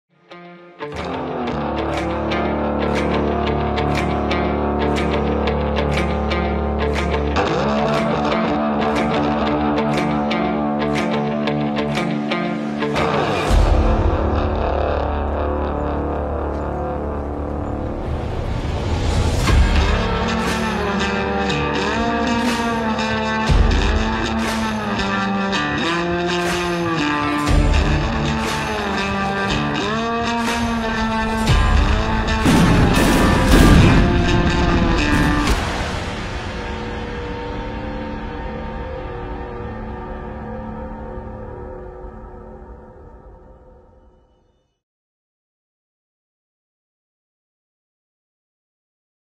ICM ‘Kozak-2’ Ukrainian MRAP-class Armored Vehicle (Item number 35014)
‘Kozak-2’
Ukrainian MRAP-class Armored Vehicle
Step by step instruction
Item number 35014 | Scale 1/35

.com/ICM.Models